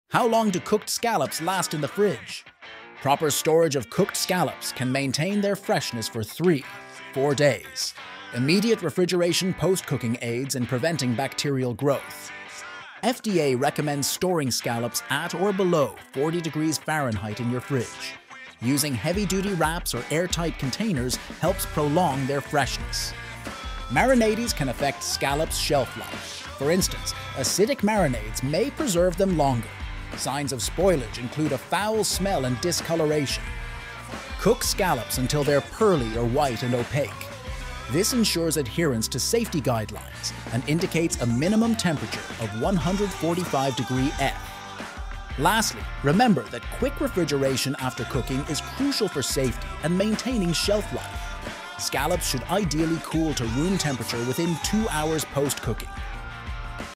Maximizing Shelf Life: Cooked Scallops in the Fridge with John Bird
I'm John Bird, a former chef and barista at the Boat Basin Cafe in New York. Today, I'm sharing key tips on preserving the freshness of cooked scallops in the fridge.